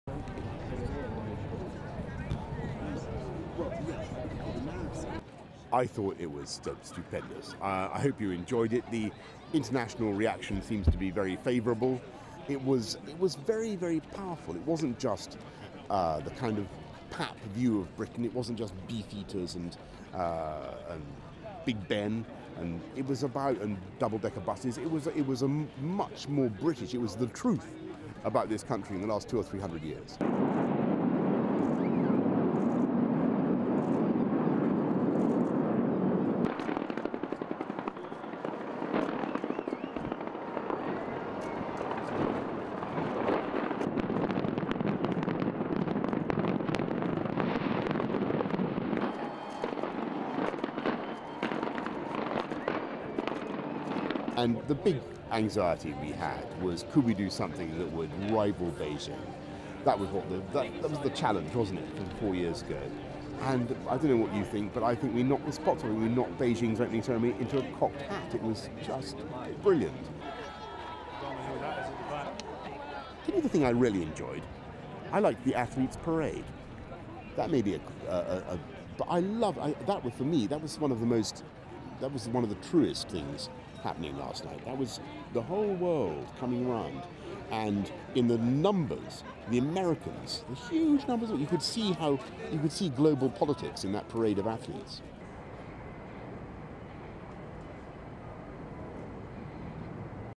London mayor on opening ceremony, Beijing; more images of Red Arrows, fireworks
(28 Jul 2012) STORYLINE
London mayor Boris Johnson said Friday's opening ceremony to the London Games had knocked Beijing's popular spectacular into "a cocked hat" .
Speaking at the Olympic Park on Saturday, the mayor described the Danny Boyle-directed four hour show as "stupendous".
"It was very very powerful, it wasn't just the kind of pap view of Britain. It wasn't just Beefeaters and Big Ben," he said.
"It was much more British! It was the truth about this country in the last two or three hundred years."
Johnson said the biggest fear of those organising the London Games was whether they could match the opening of the 2008 Games in Beijing.
"That was the challenge, wasn't it, from four years ago," he said,  "and, I don't know what you think, but I think we knocked the spots off it. We knocked Beijing's opening ceremony into a cocked hat. It was just brilliant."
The mayor said one aspect of the ceremony that he particularly enjoyed had been the parade of athletes.
"That for me, that was one of the most, that was one of the truest things, happening last night. That was the whole world coming round and in the numbers. The Americans - huge numbers of them - you could see global politics in that parade of athletes," he said.